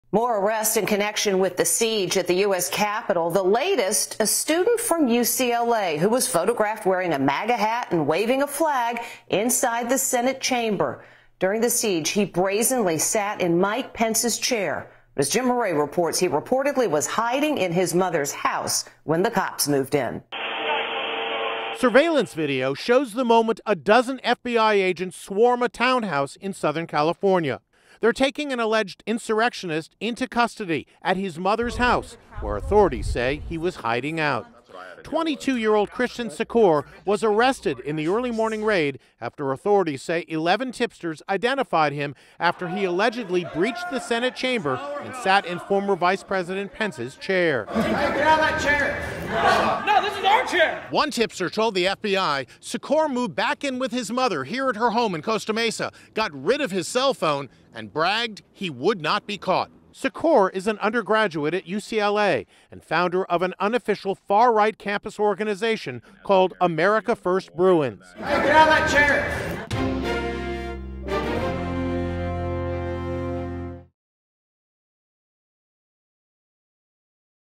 22-Year-Old Arrested at Mom’s Home Following Capitol Attack
A UCLA student photographed wearing a MAGA hat and waving a flag inside the Senate chamber during the Jan. 6 Capitol riots has been arrested. Christian Secor, 22, was taken into custody by about a dozen FBI agents in an early morning raid at his mother’s house, where authorities say he was hiding out. Authorities say 11 tipsters identified Secor after he allegedly breached while live streaming the attack on the Senate chamber, and sat in former Vice President Pence’s chair.